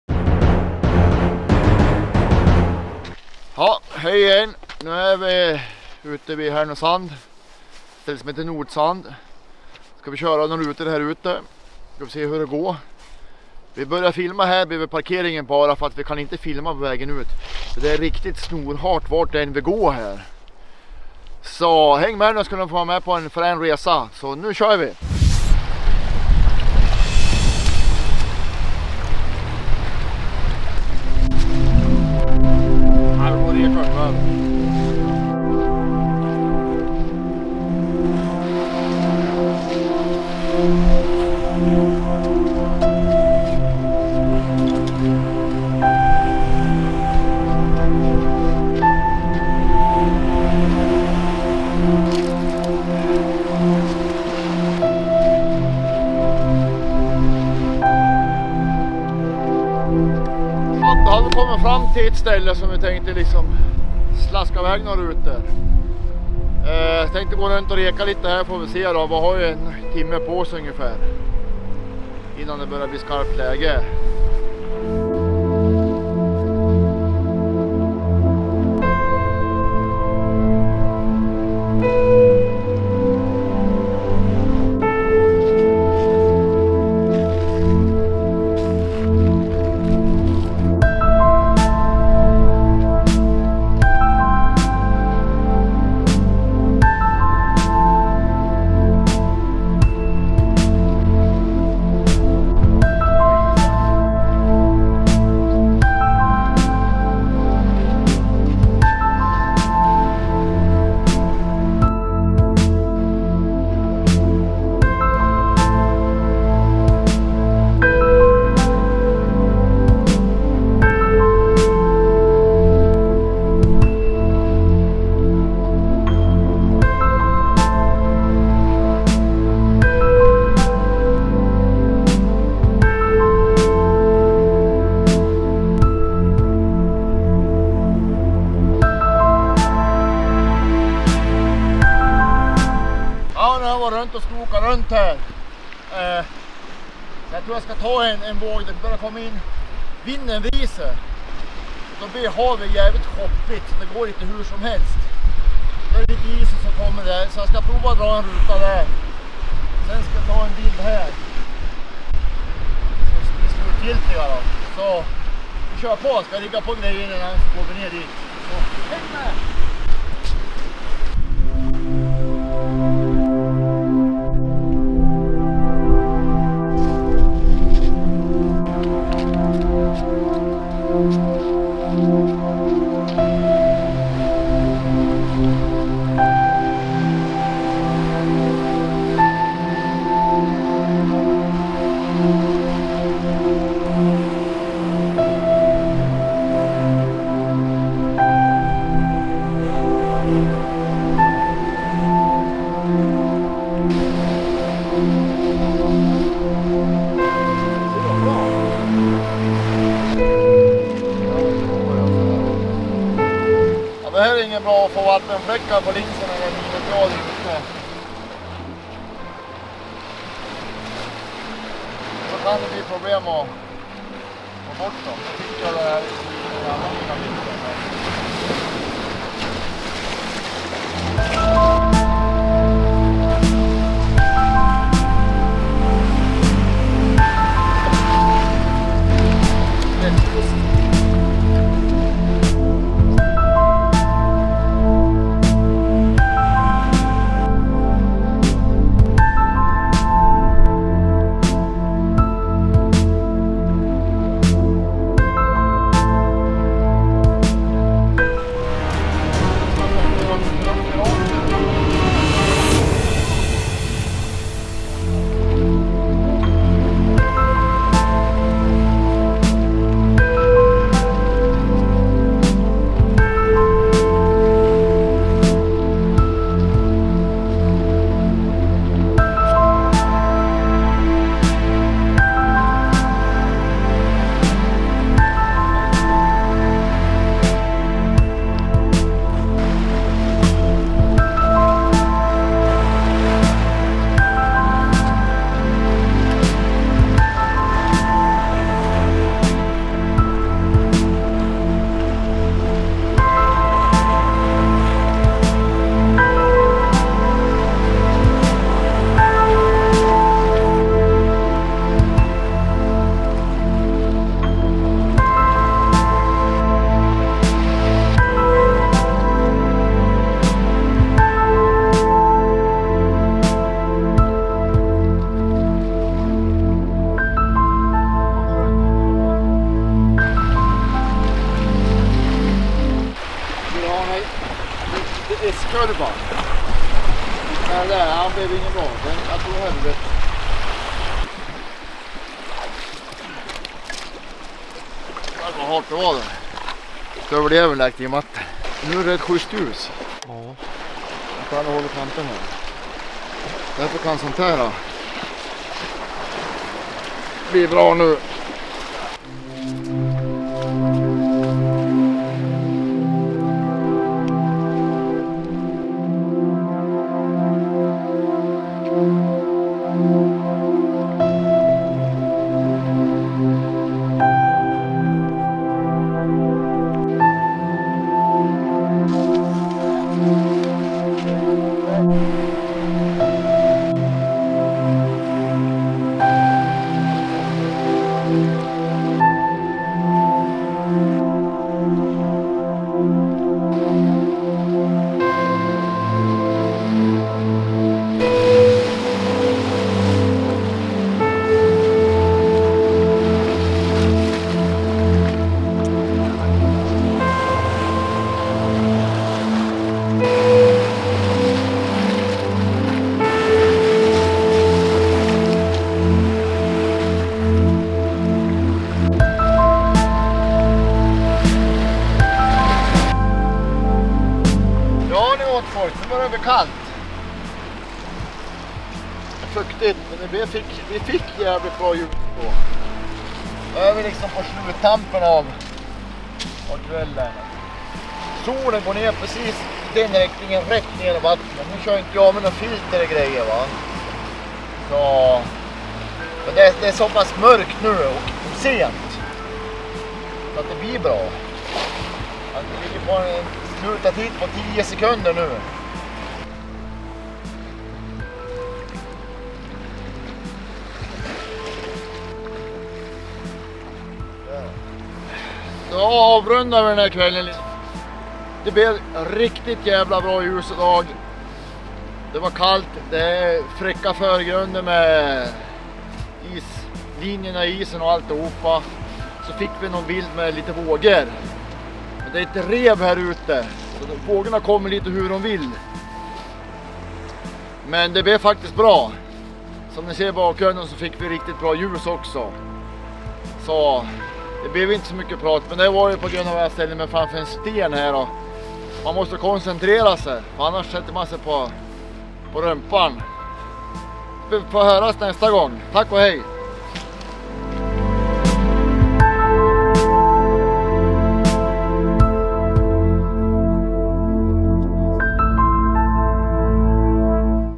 Isiga klippor i Notsand
Följ med fotograf Tomas Spåra på fotoäventyr när han fotar bland annat landskap, sjöar, natur kust och hav i en serie dokumentärfilmer.